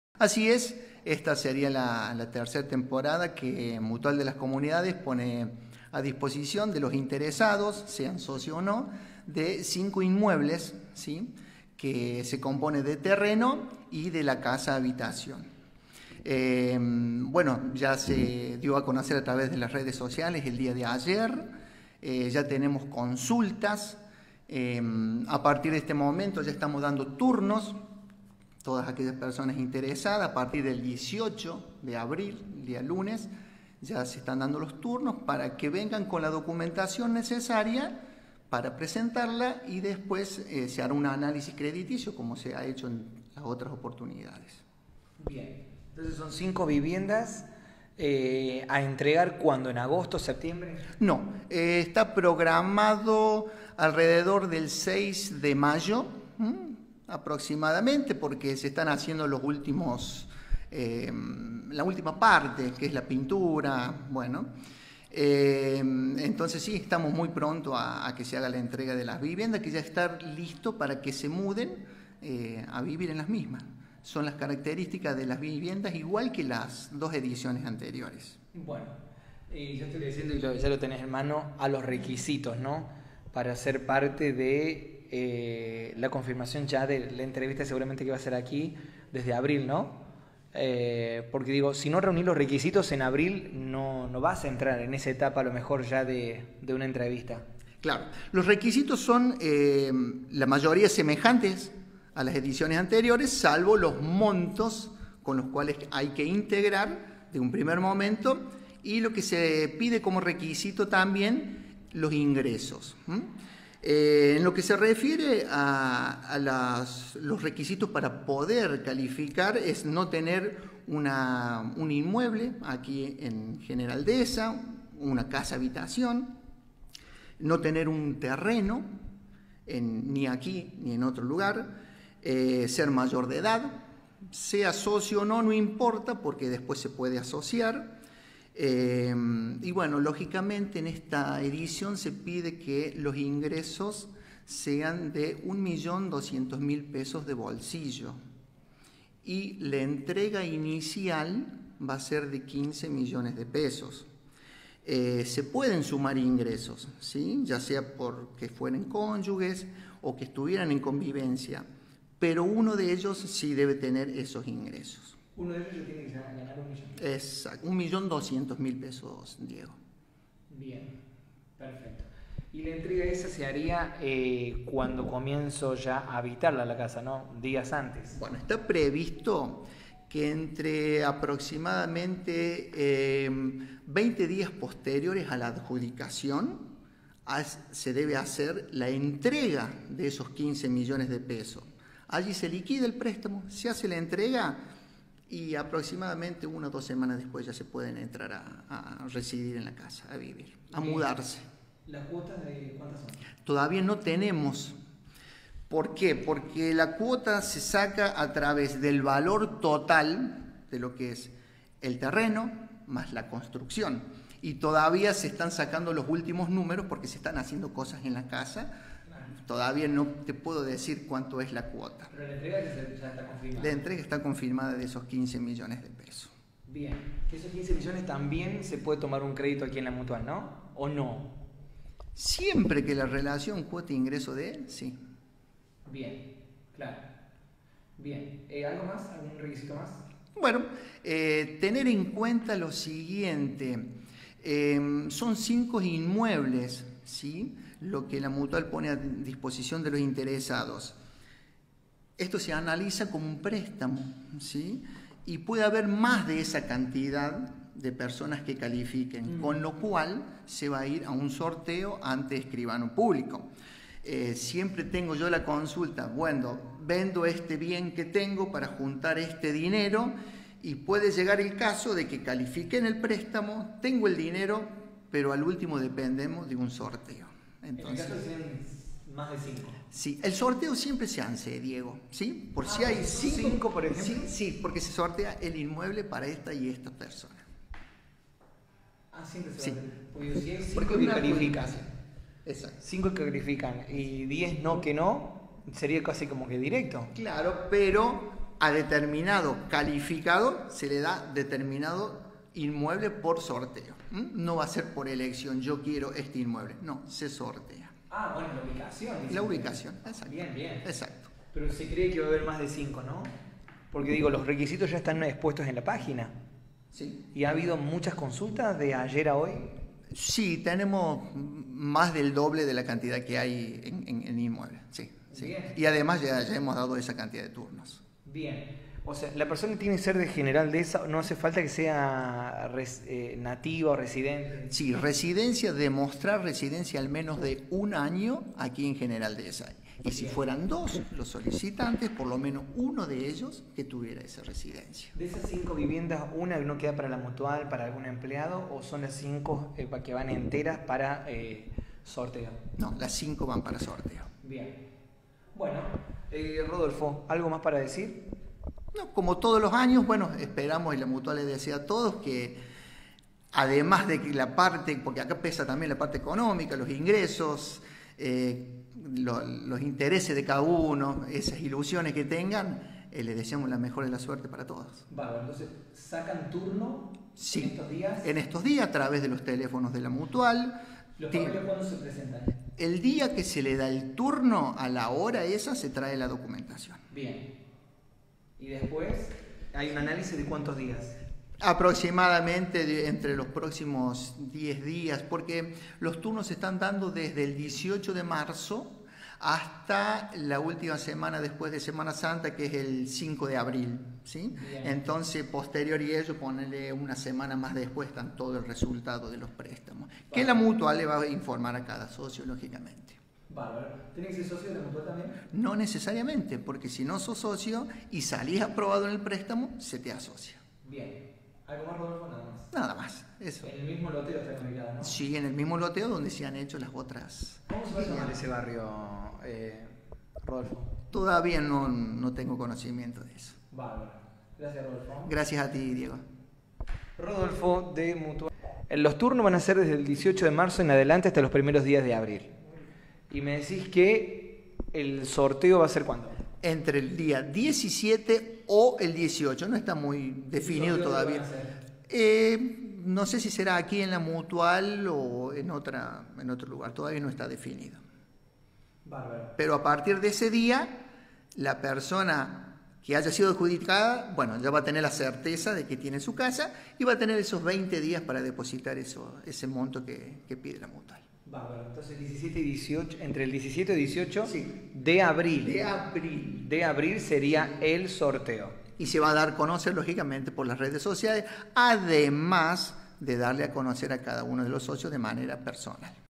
GENERAL DEHEZA: “Mutual de las comunidades” abre inscripciones para adjudicar terreno + vivienda.
“Mutual de las comunidades” abre inscripciones para adjudicar terreno + vivienda lista para habitar, Rodolfo Caudana con requisitos para la convocatoria.
“Estas viviendas, pensadas para brindarte un hogar completo, se entregarán listas para habitar y contarán con todos los servicios necesarios. La adjudicación se realizará mediante un sorteo presencial, garantizando transparencia, y será llevado a cabo por un Escribano público entre todos los preseleccionados que cumplan con los requisitos. Para obtener más información o para agendar tu turno y presentar la documentación, te esperamos en nuestra filial de General Deheza, ubicada en Juan José Paso 35. También poidés comunicarte con nosotros al (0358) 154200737”, comunicó la Mutual en sus redes oficiales.